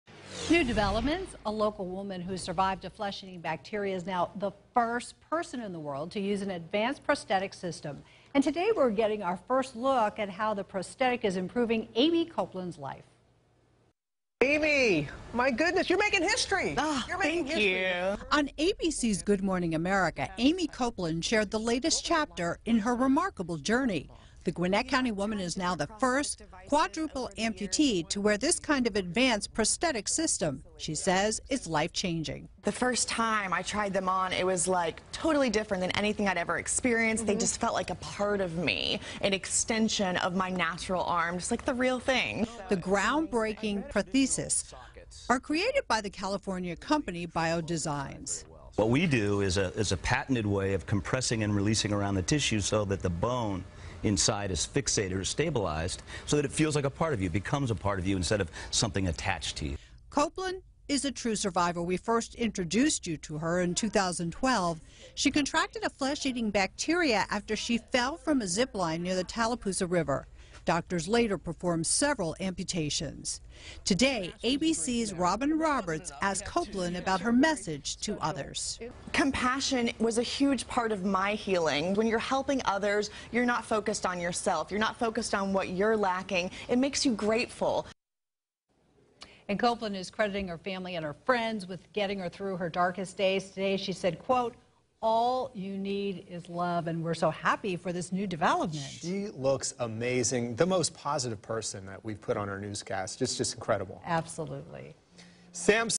Woman who survived flesh eating bacteria uses an advanced prosthetic system
Aimee Copeland is the first quadruple amputee to use this advanced system.